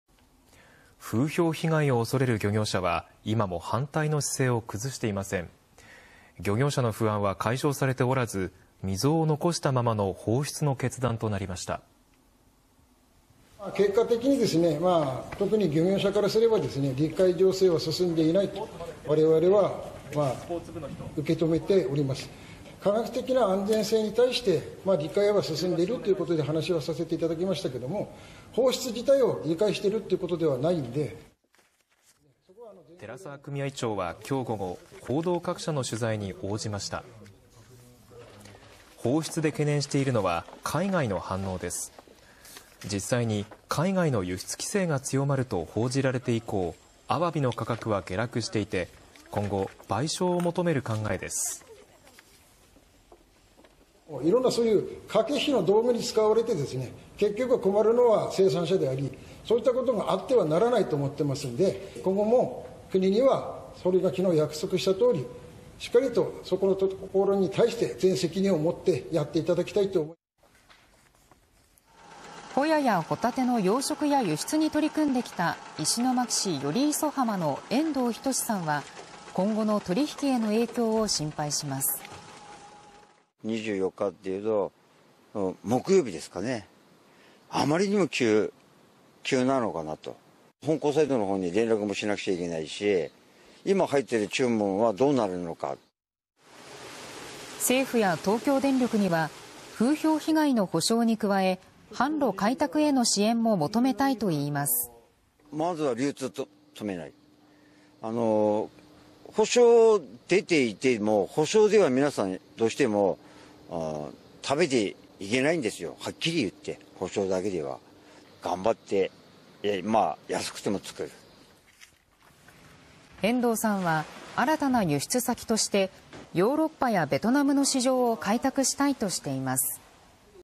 「理解醸成は進んでいない」処理水海洋放出をめぐり宮城県の漁業者
漁業者の不安は解消されておらず、溝を残したままの放出の決断となりました。